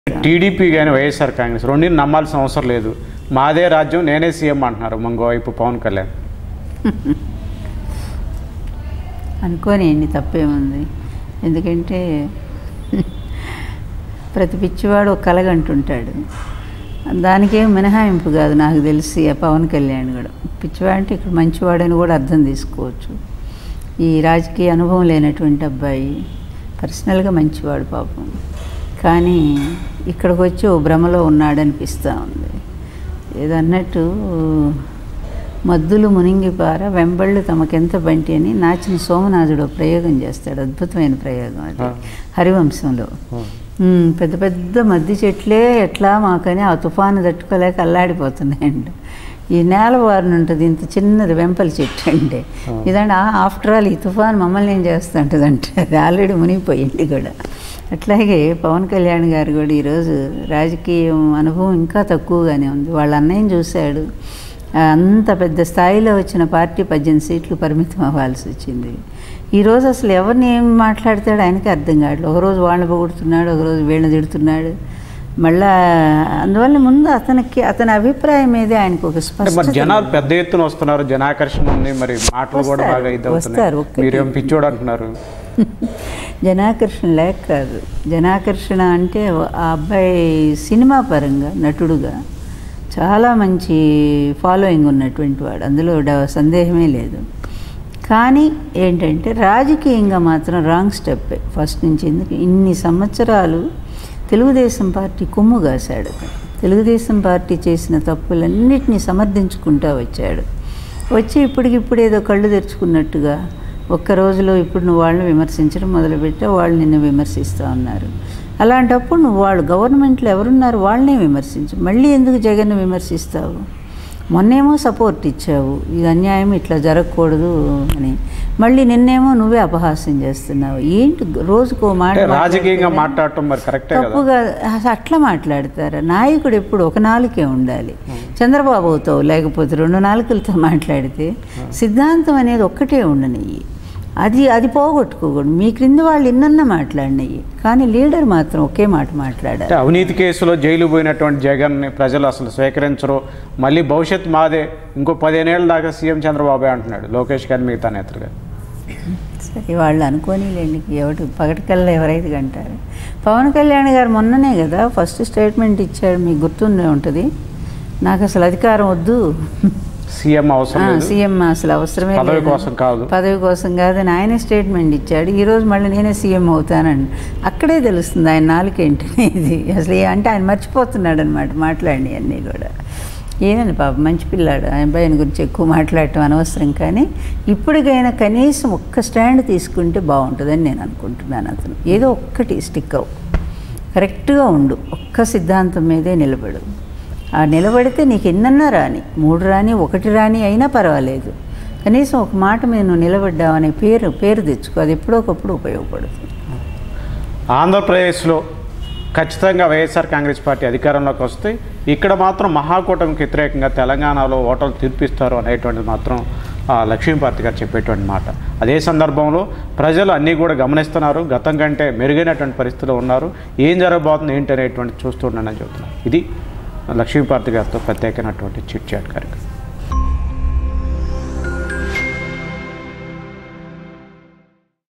Lakshmi Parvathi About Pawan Kalyan Janasena | AP 2019 Elections | TV90 News Telugu
LakshmiParvathi About PawanKalyan Janasena Contesting In AP Assembly 2019Elections In Her Latest Exclusive Interview With TV90 News Telugu.